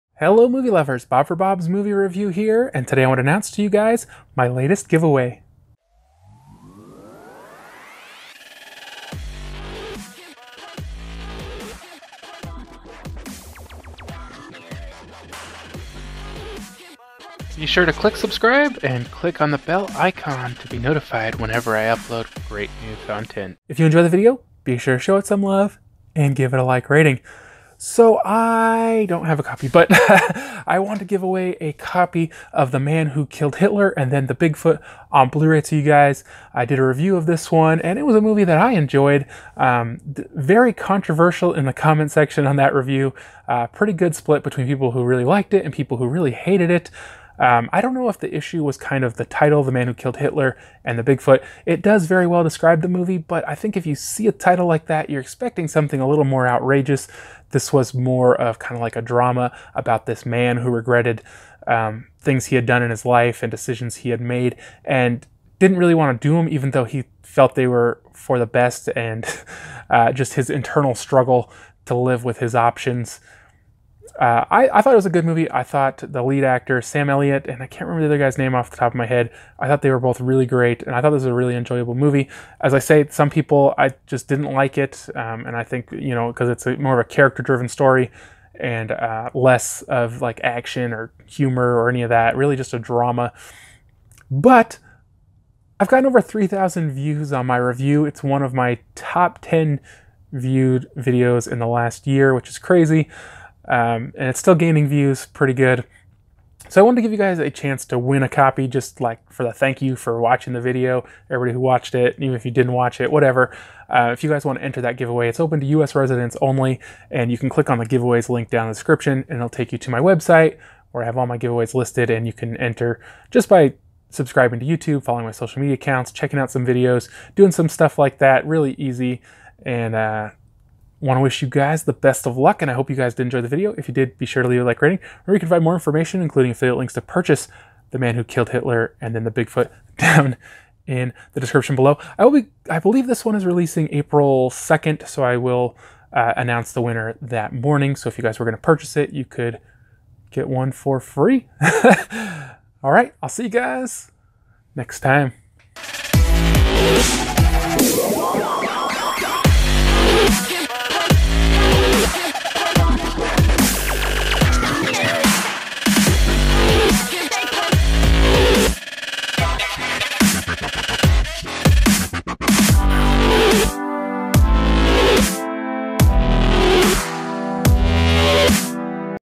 The Man Who Killed Hitler and Then The Bigfoot Blu Ray Giveaway
- The Man Who Killed Hitler and Then The Bigfoot
Adventure, Drama
Rated Not Rated
Run-Time 1H 38M
Rating 6.1/10
Bob’s Rating 7/10
Director Robert D. Krzykowski
Stars Sam Elliott, Aidan Turner, Ron Livingston, Caitlin FitzGerald, Sean Bridgers, Ellar Coltrane, Larry Miller, Anastasia Tsikhanava, Mark Steger, Rizwan Manji, Silas Archer Gustav, Kelley Curran, Nikolai Tsankov, Alton Fitzgerald White, Mark Lund